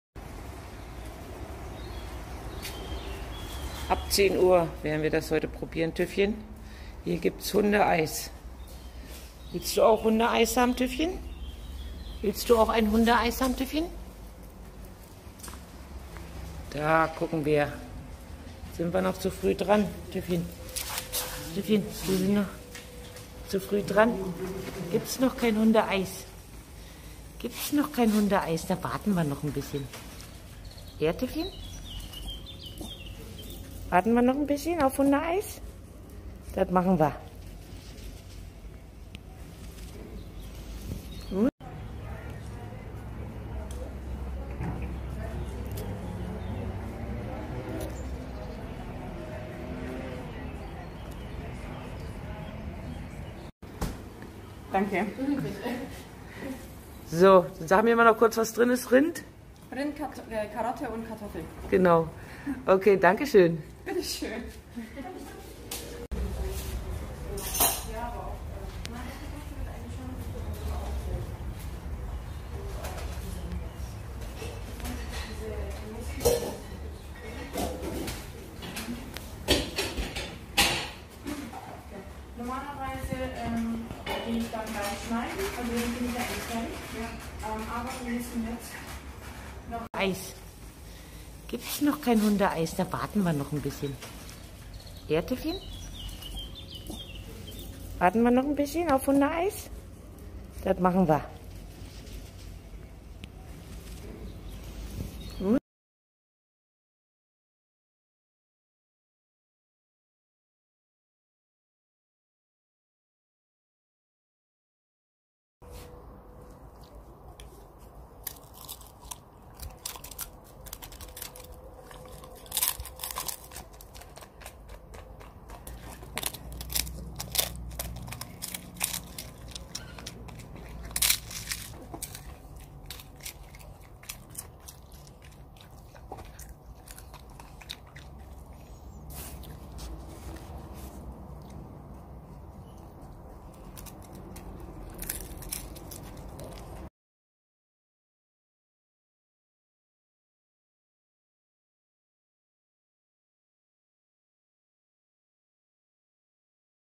Ice cream for dogs Test Hundeeis
Bei dem heissen Wetter habe ich Hundeeis getestet.